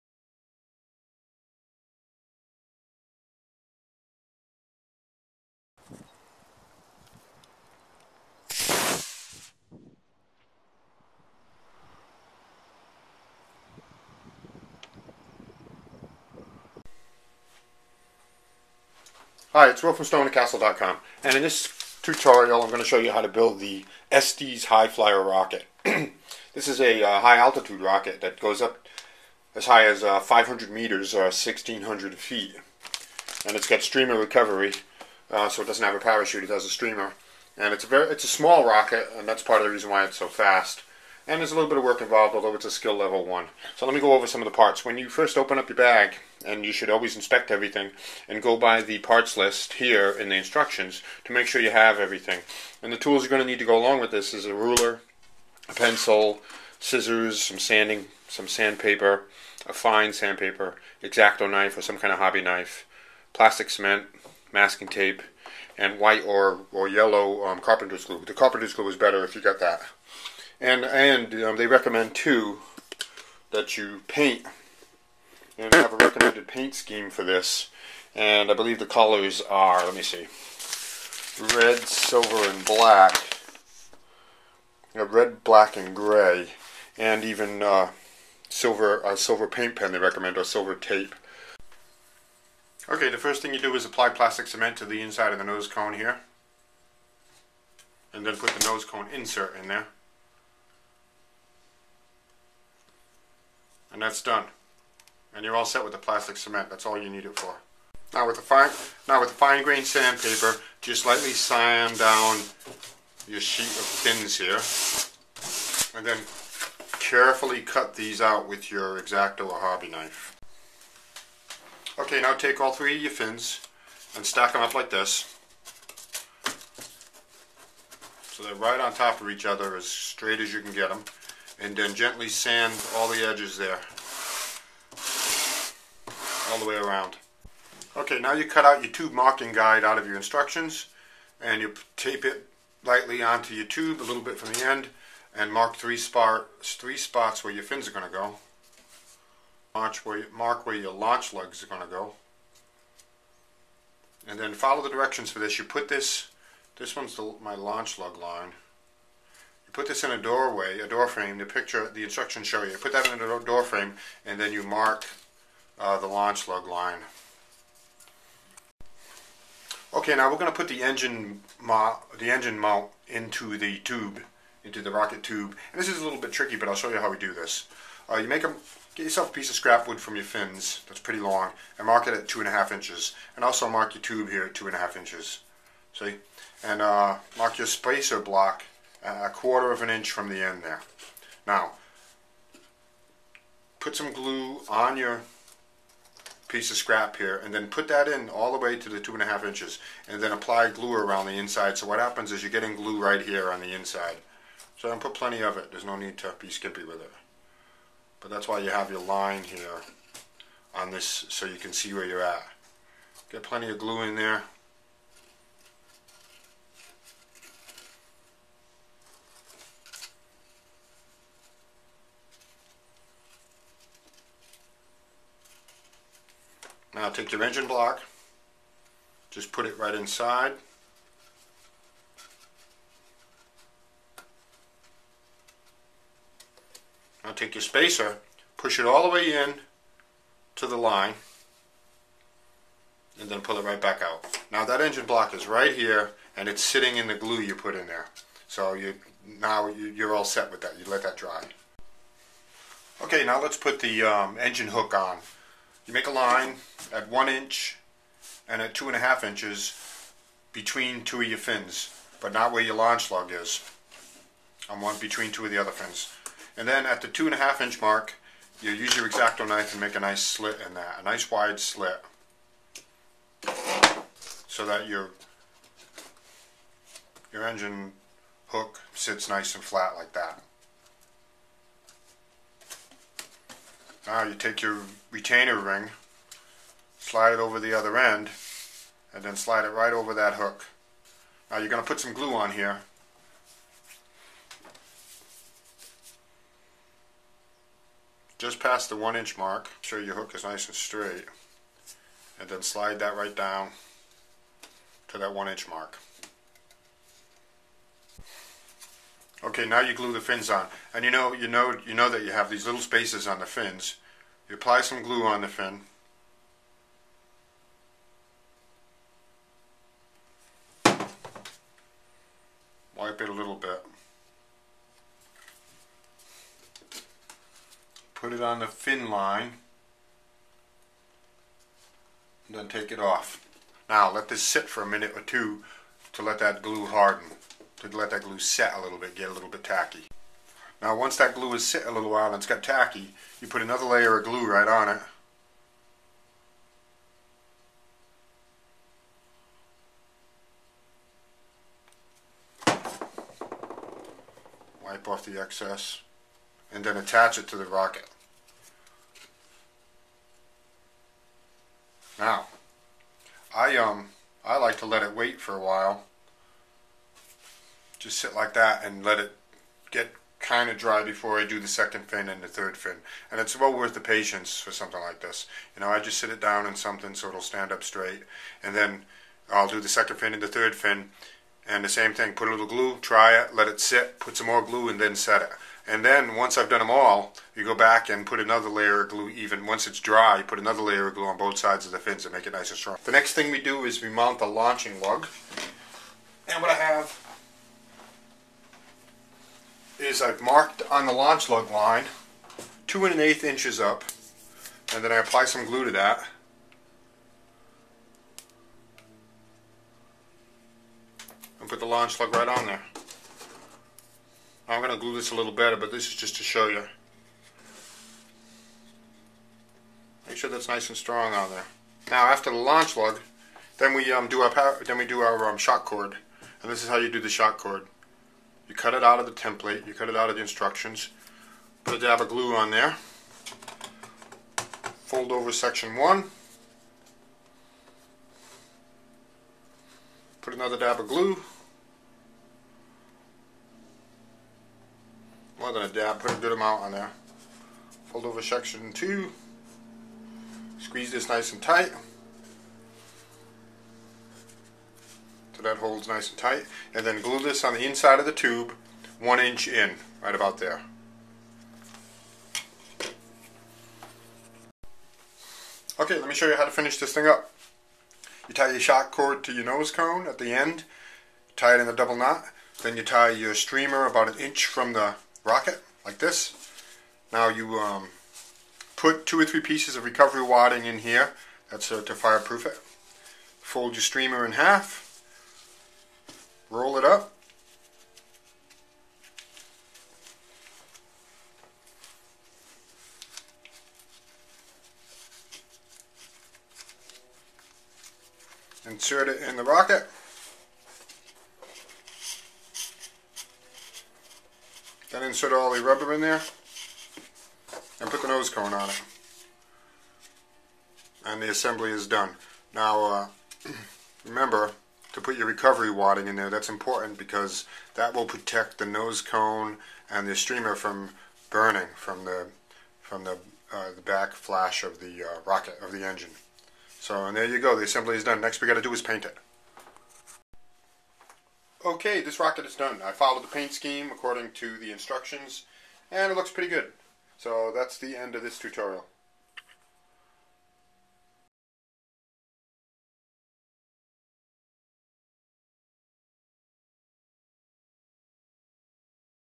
Hi Flier Model Rocket Launch and Assembly
This is a fast little rocket that flies over 500 meters into the air. This video shows the launch and assembly. Great little model rocket for beginners who want to build a complete rocket that flies real high and real fast.

About my Channel:
I have hundreds of videos covering all kinds of creative projects.